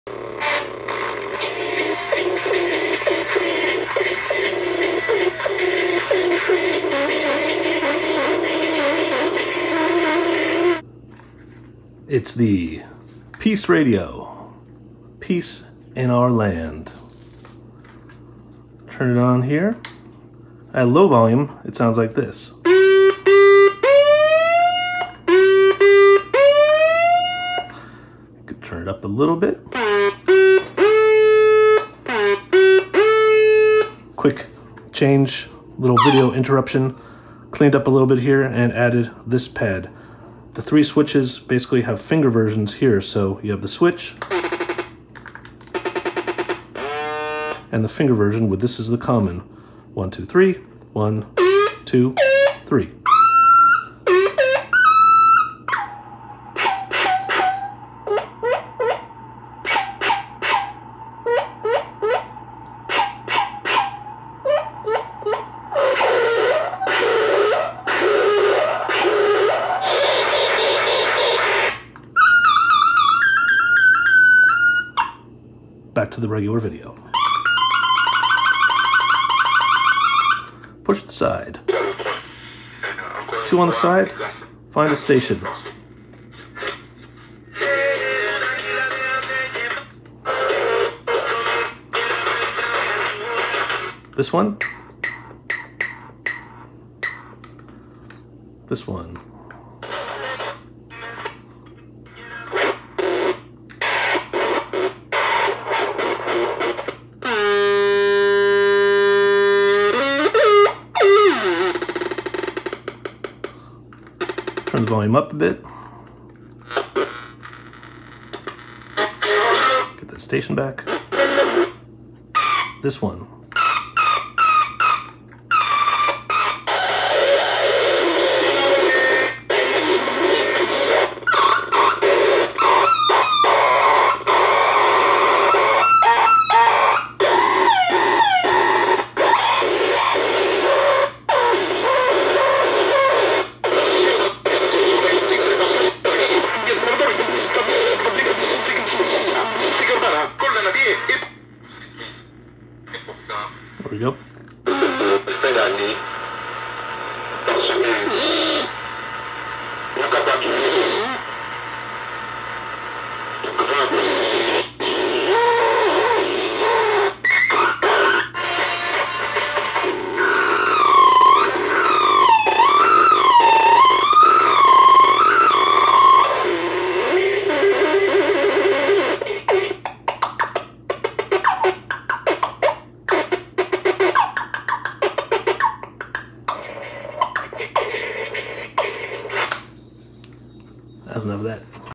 Circuit Bending a Vintage Transistor Radio - The Peace Radio "Synthesizer" Thingy
Check out a correct aspect ratio version of this (I don't know what I was thinking here. And I'm a video editor)

I used to make these things in the 70s when I was in elementary school and thought I'd try it again. I had no idea until just recently there was a name for this kind of thing. I guess I was an early pioneer?